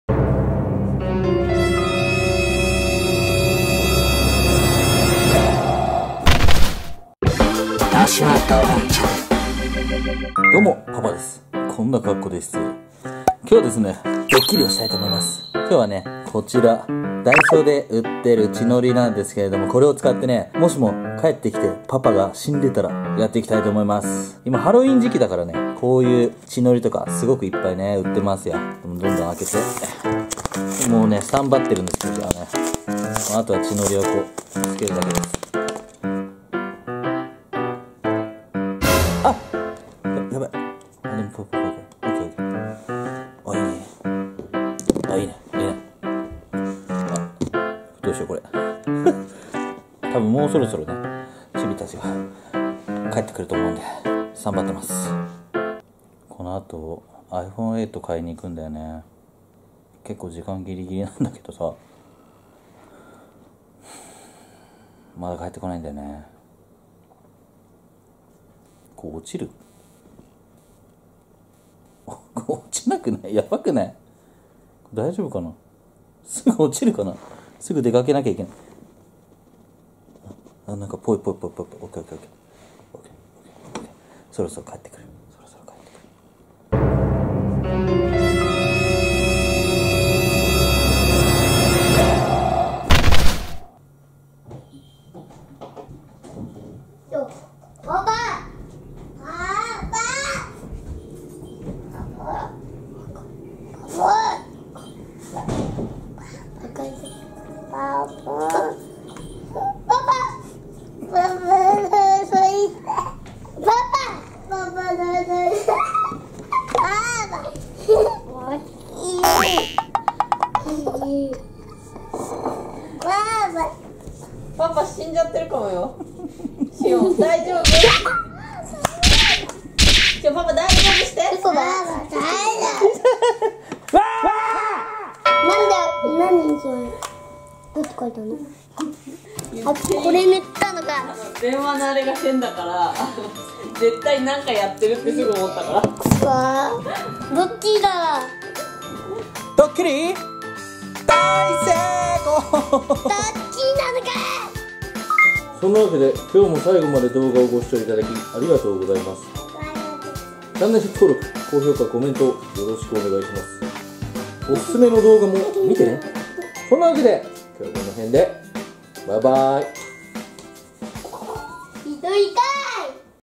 【ドッキリ】もしもパパが血を出して倒れてたら…子供たちの反応は！？
◆お手紙・プレゼントなどはこちらに送ってください↓
〒106-6137